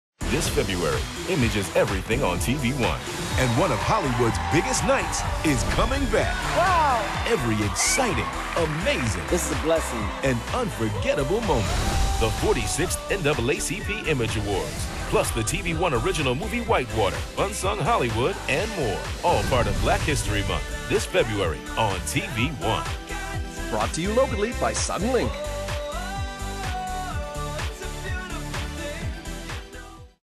Black History Month Programming on TV One
Celebrate Black History Month with documentaries, shorts, specials and movies playing on TV One throughout February.